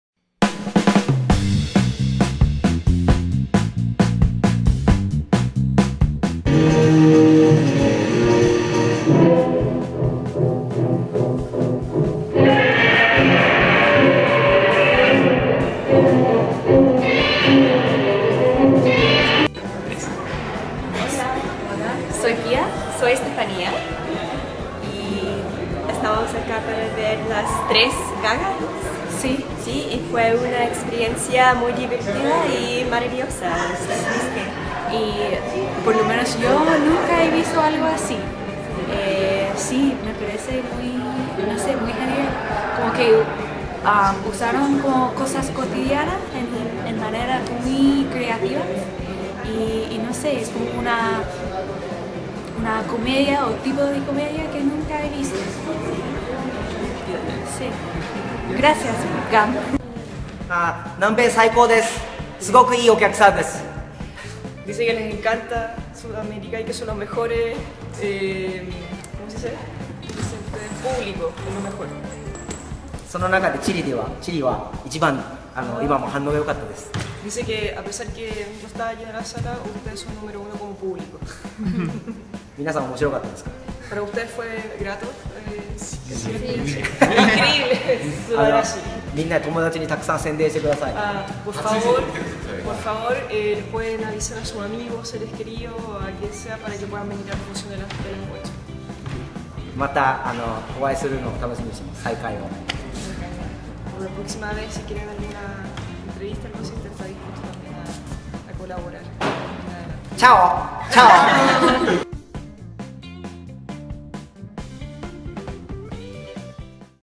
"3 Gaga Heads" en Chile
Jóvenes Críticos estuvo en la visita del trio japones "3 Gaga Heads" en Chile. Al finalizar la función pudimos conversar con el publico asistente y parte del divertido elenco de esta performance.